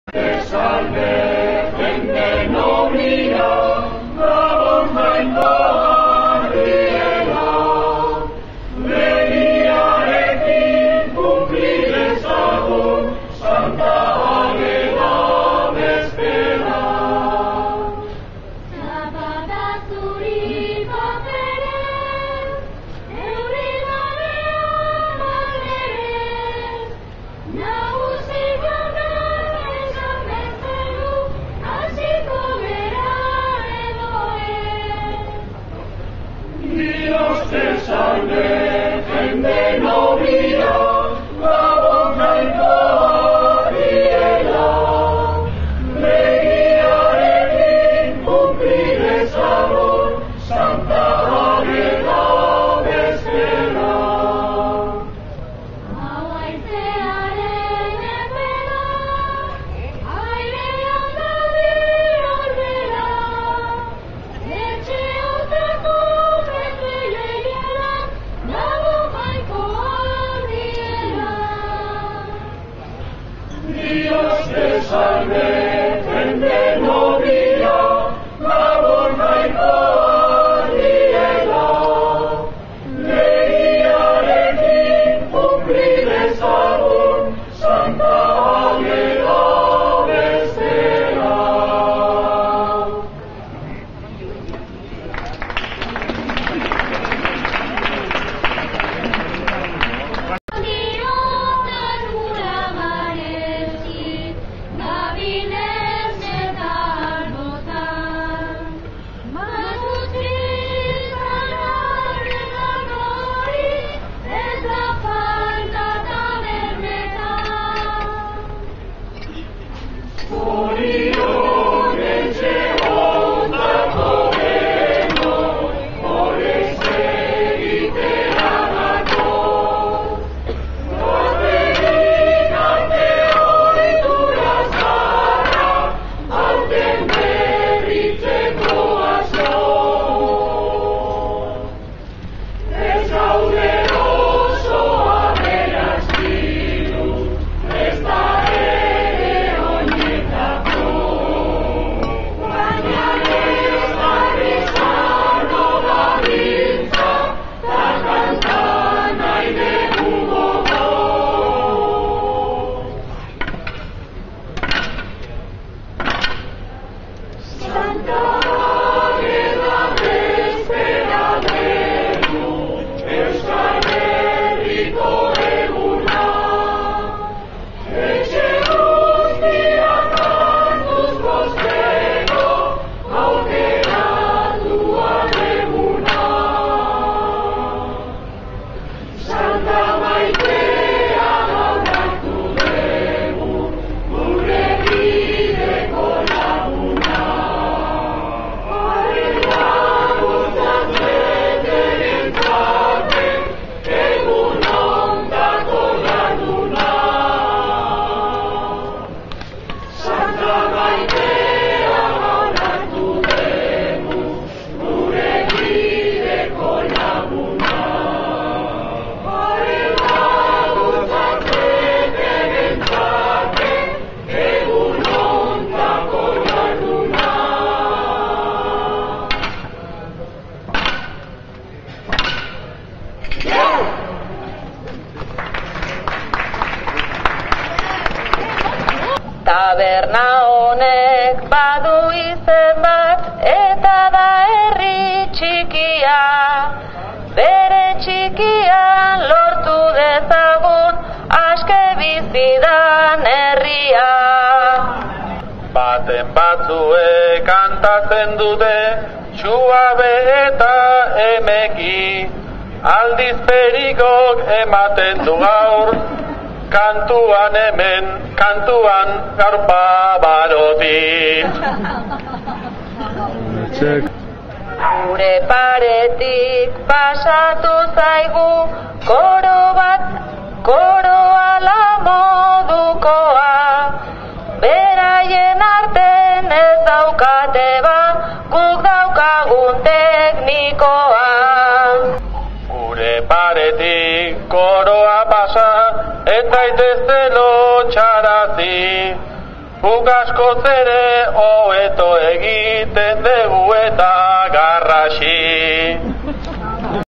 Ageda bezpera Donostia 2009
Ageda bezpera 2009-2-4. Puro koplak abesten.